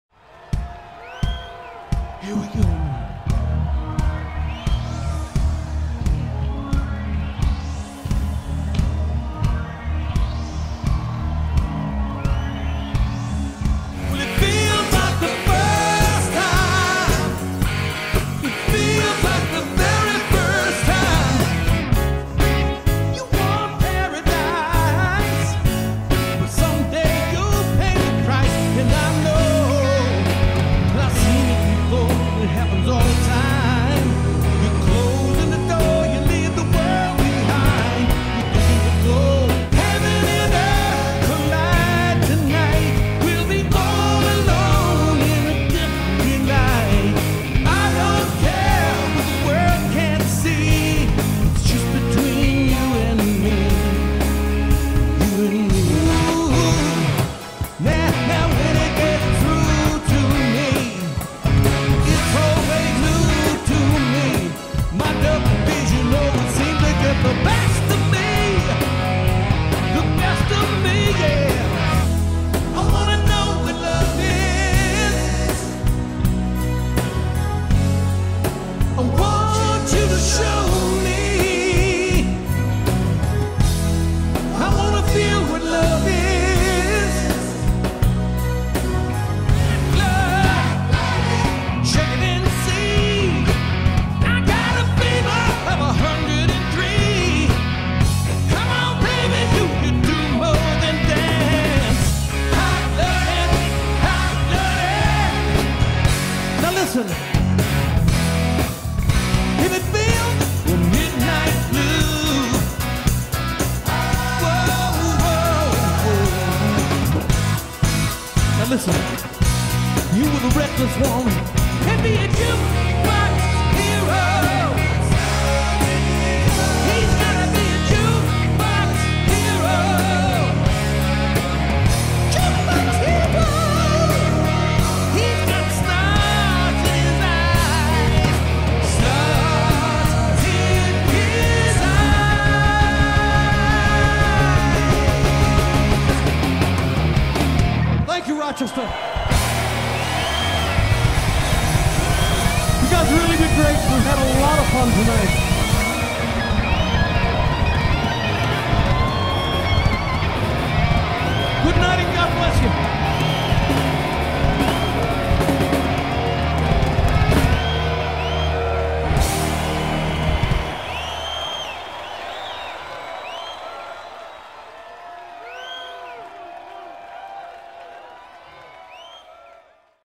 LOU GRAMM 2012 TEASER 3 MIN
Lou Gramm plays live in Rochester, NY.
Special thanks to WXXI and WCMF.